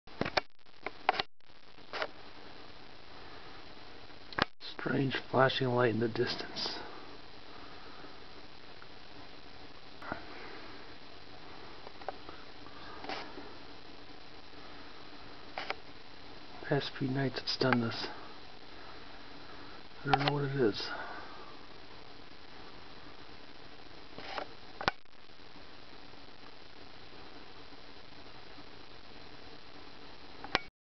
flashing light
Some flashing white light seen in the distance. Some nights it's on and flashing, other times it's off. Not really sure what it's used for.